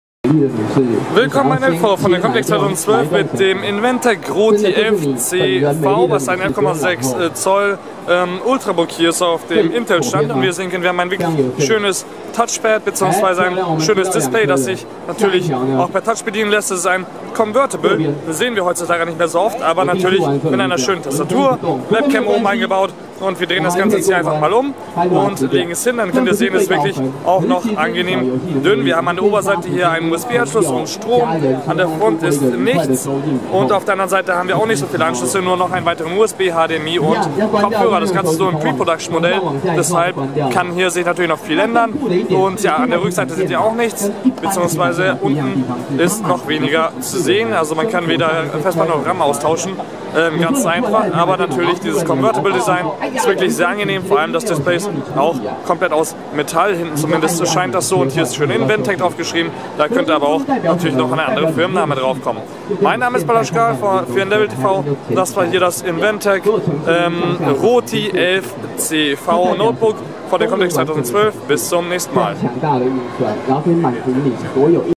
Inventec CRO-TFCV Convertible Ultrabook im Hands On
- Inventec Ultrabook mit Konvertible Display im Hands on auf der Computex 2012.